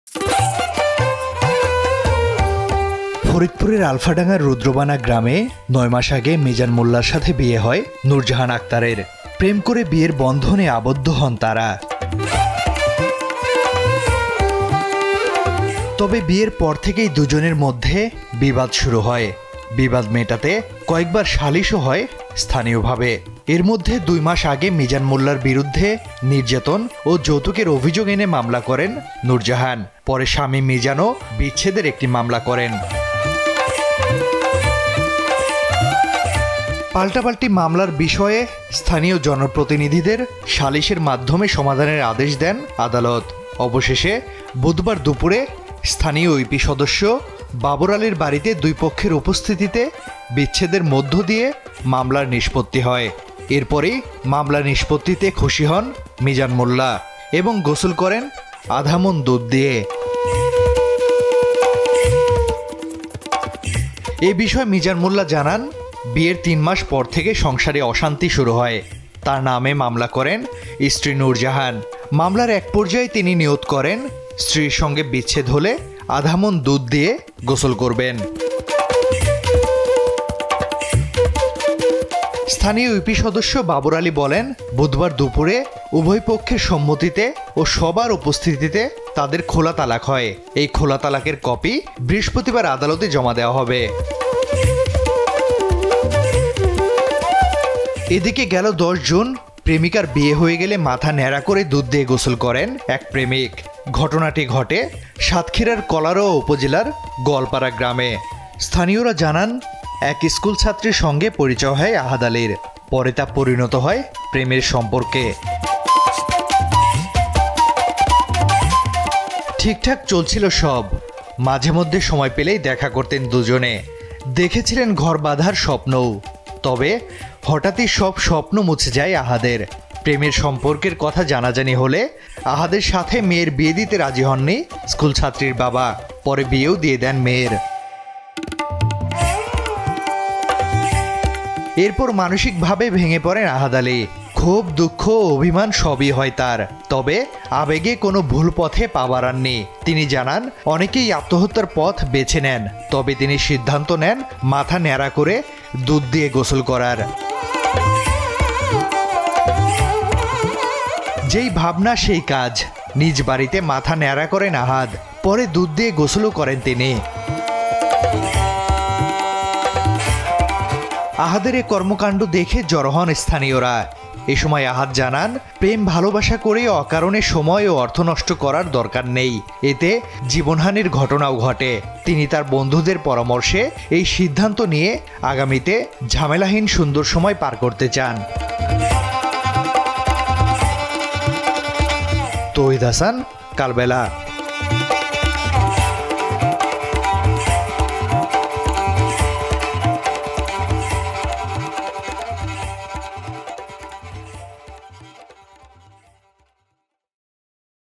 প্রেমের বিয়ের বিচ্ছে'দ, ২০ কেজি দুধ দিয়ে গোসল যুবকের | Milk Bath | Love Marriage | Faridpur|Kalbela
ফরিদপুরের আলফাডাঙ্গার রুদ্রবানা গ্রামে ৯ মাস আগে মিজান মোল্যার সাথে বিয়ে হয় নুরজাহান আক্তারের। প্রেম করে বিয়ের বন্ধনে আবদ্ধ হন তারা। তবে বিয়ের পর থেকেই দুজনের মধ্যে বিবাদ শুরু হয়। বিবাদ মেটাতে একাধিকবার সালিসও হয় স্থানীয়ভাবে। এরমধ্যে দুই মাস আগে মিজান মোল্যার বিরুদ্ধে নির্যাতন ও যৌতুকের অভিযোগ এনে মামলা করেন নুরজাহান। পরে স্বামী মিজানও বিচ্ছেদের একটি মামলা করেন।